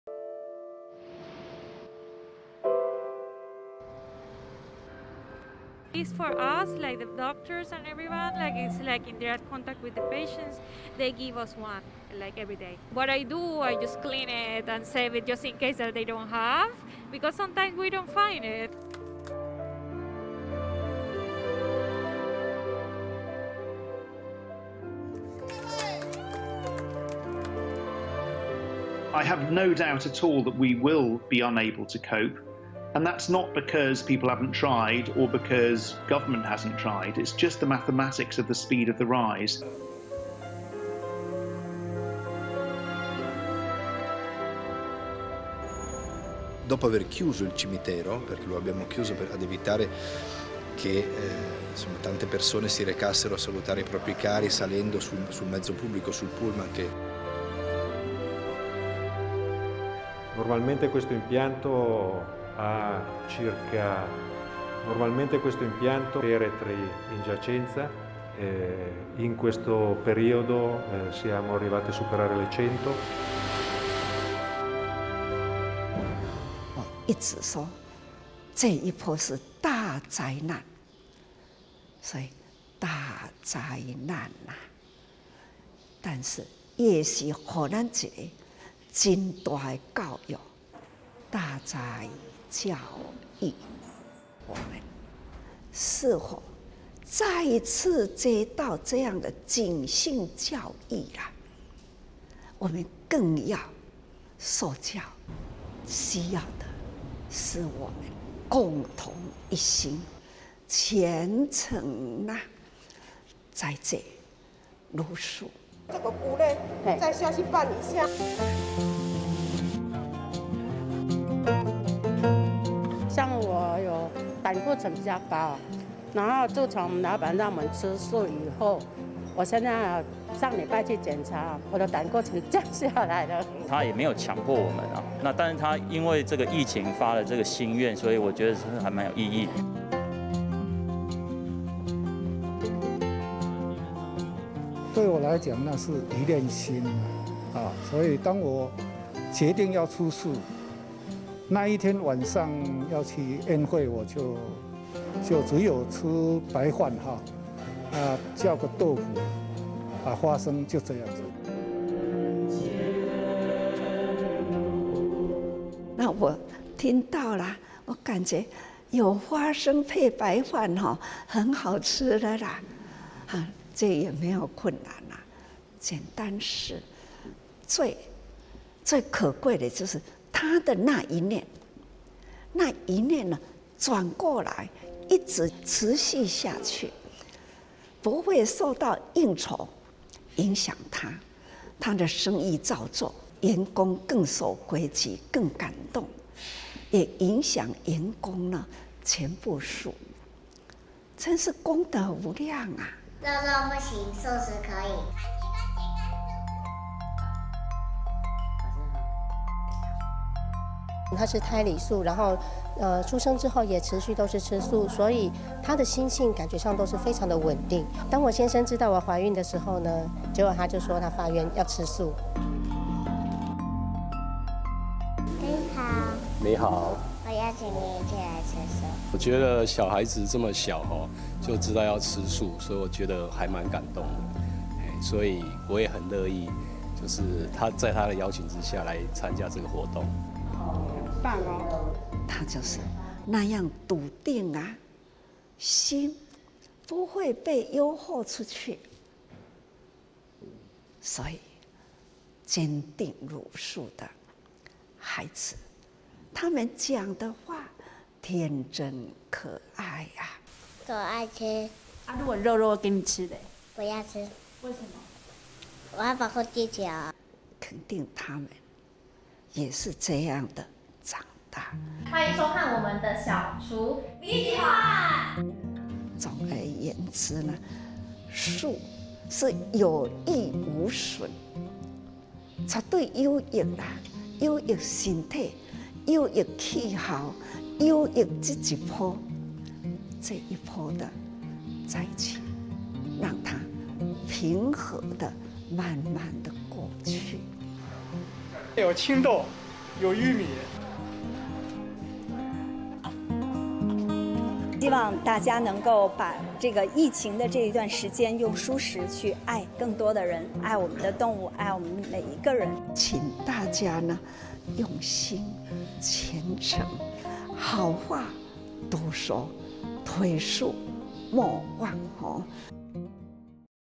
20200331【志工早會】大哉教育 非素不可
關切全球疫情。今天志工早會，證嚴上人呼籲，這一波是大災難，也是給我們一個很大的教育。對慈濟這一波，也有很大的挑戰。發揮愛心同時，要做好自好保護，最好的應對方式就是齋戒茹素，讓疫情平和的過去。

紐約 艾姆赫斯特醫院醫師 Barbara Porelo：「我們每天需要面對病人，醫院給我們醫師與護理師，一天給我們一個，我有時候會把口罩清潔一下，以防萬一真的不夠的時候，因為有時候很難找到口罩可用。」

倫敦醫院急診室 醫生：「毫無疑問 我們將無法應付病患，並不是因為人民或政府不努力，只是病例數增加太快。」

貝加莫市長 戈里：「我們已經關閉了公墓，避免太多人搭公車前來，與死去的親友告別。」

火葬場人員 馬尼內洛：「這裡可以容納30具棺材，但這期間達到100多具，我們每天火化24具棺材，但今天已經火化40具了。」

證嚴上人開示：「我一直說這一波是大災難，所以大災難，但是也是給我們一個很大的教育。大哉教育。我們是否再次接到這樣的警訊教育，我們更要受教。需要的是我們共同一心，虔誠齋戒、茹素。」

工廠員工 王秀梅：「像我膽固醇比較高，然後自從老闆讓我們吃素以後，我現在 上禮拜去檢查，我的膽固醇降下來了。」

董事長林坤旺的兒子 林義順：「他也沒有強迫我們，但他因為這個疫情發了這個心願，所以我覺得還滿有意義。」

工廠董事長 林坤旺：「對我來講那是一念心，所以 當我決定要吃素，那一天晚上要去宴會，我就只有吃白飯，叫個豆腐 花生就這樣子。」

證嚴上人開示：「我聽到了，我感覺有花生配白飯很好吃了，這也沒有困難，簡單事。最可貴的就是他的那一念，那一念轉過來，一直持續下去，不會受到應酬影響到他。他的生意照做，反而員工更守規矩，更感動，也影響員工全吃素，真是功德無量。」

小蔬童 蘇晁賢：「肉肉不行 素食可以。」

蘇晁賢媽媽 陳靜儀：「他是胎裡素，出生之後也持續都是吃素，所以他的心性，感覺上都是非常地穩定，當我先生知道我懷孕的時候，結果他就說他發願要吃素。」

小蔬童 蘇晁賢：「你好 ，我邀請你一起來吃素。」

民眾 饒國清：「我覺得小孩子這麼小，就知道要吃素，我覺得還滿感動的，所以我也很樂意在他邀請下，來參加這個活動。」

證嚴上人開示：「他就是那樣篤定了心，不會被誘惑出去，所以堅定茹素的孩子，他們講的話天真可愛，肯定他們也是這樣的長大。」

證嚴上人開示：「總而言之素是有益無損，絕對有益有益身體，有益氣候，有益這一波，這一波的災情，讓它平和地慢慢地過去。」

北京慈濟志工 王智慧：「希望大家能夠把疫情這段時間，用蔬食去愛更多人，去愛我們的動物，愛我們每一個人。」

證嚴上人開示：「請大家呢用心，虔誠，好話多說，推素莫忘。」
馬來西亞分會